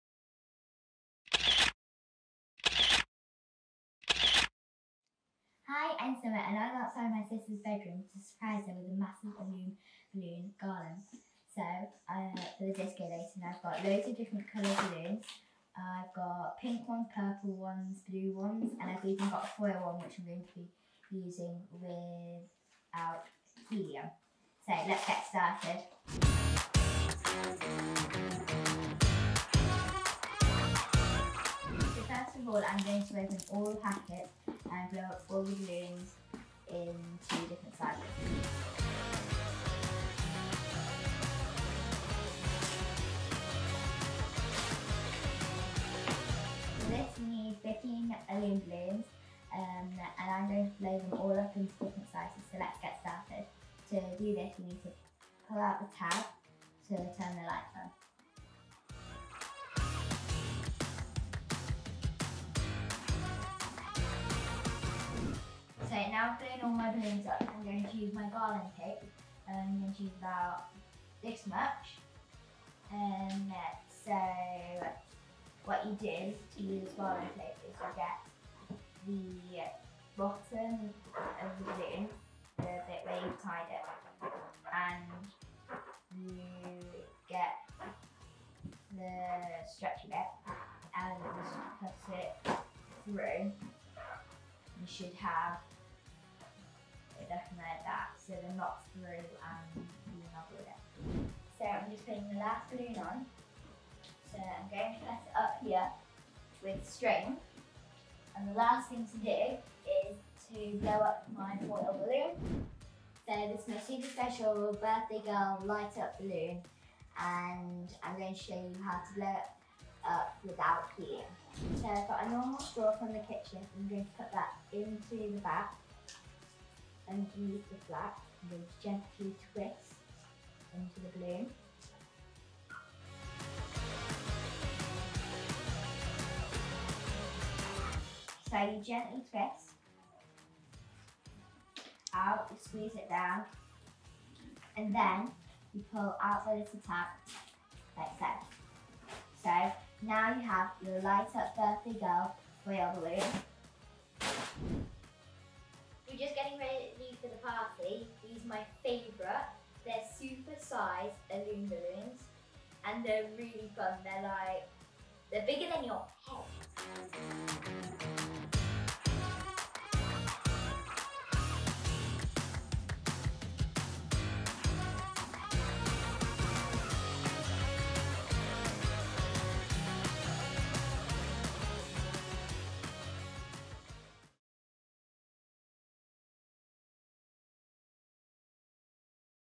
The ulitmate birthday girl decorations from illooms LED balloons
Are you planning for your birthday girl's special day?

Make the birthday decorations extra special with illooms LED balloons.